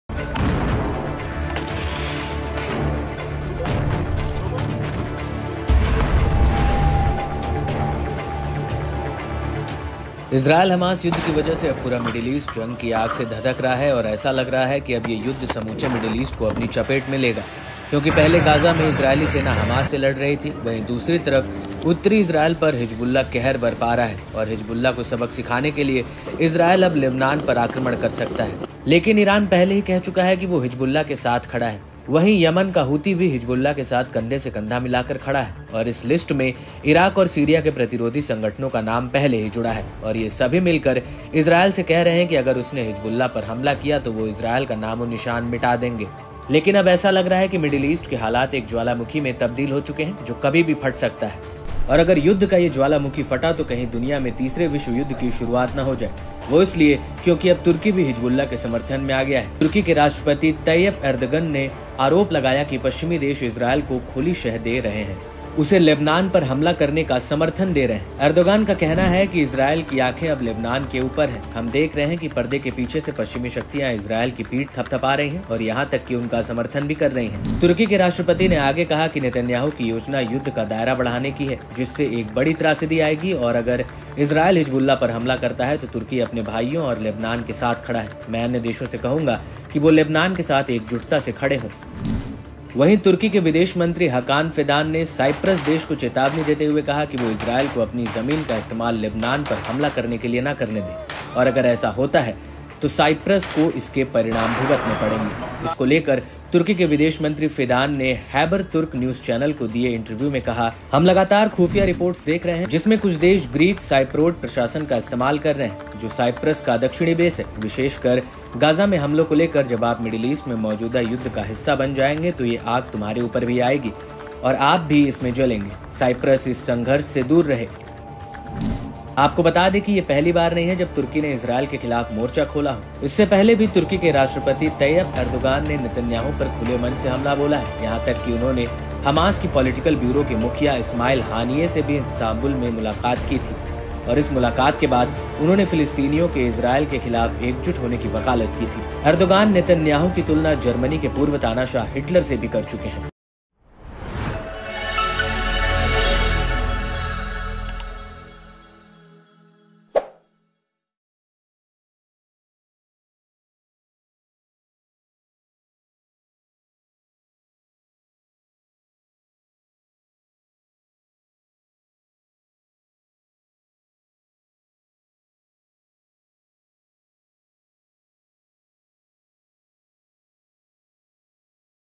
Israel Hamas War | इज़रायल हमास युद्ध के बीच Turkey की Entry, होने वाला विश्वयुद्ध !Netanyahu | N18G
Amidst the Israel Hamas War, the situation of Israel Lebanon War is developing. Regarding this, Turkey President Tayyip Erdogan has said that Western countries are openly supporting Israel to attack Lebanon. Erdogan says that Israel's eyes are now on Lebanon. We are seeing that from behind the scenes, Western powers are patting Israel's back and even supporting them. Turkey President Tayyip Erdogan further said that PM Netanyahu's plan is to expand the scope of the war which will lead to a big tragedy. And if Israel attacks Hezbollah, then Turkey stands with its brothers and Lebanon. I would ask other countries to stand in solidarity with Lebanon.

Israel Hamas War के बीच Israel Lebanon War के हालात बनते जा रहे हैं. इसको लेकर, Turkey President Tayyip Erdogan ने कहा है कि पश्चिमी देश Israel को खुली शह देकर उसे लेबनान पर हमला करने का समर्थन दे रहे हैं. Erdogan का कहना है कि Israel की आंखे अब लेबनान के ऊपर हैं हम देख रहे हैं कि पर्दे के पीछे से पश्चिमी शक्तियां इजराइल की पीठ थपथपा रही हैं और यहां तक ​​कि उनका समर्थन भी कर रही हैं. Turkey president Tayyip Erdogan ने आगे कहा कि PM Netanyahu की योजना युद्ध का दायरा बढ़ाने की है जिससे एक बड़ी त्रासदी आएगी. और अगर Israel Hezbollah Attack करता है तो Turkey अपने भाइयों और लेबनान के साथ खड़ा है. मैं अन्य देशों से कहूंगा कि वो लेबनान के साथ एकजुटता से खड़े हों.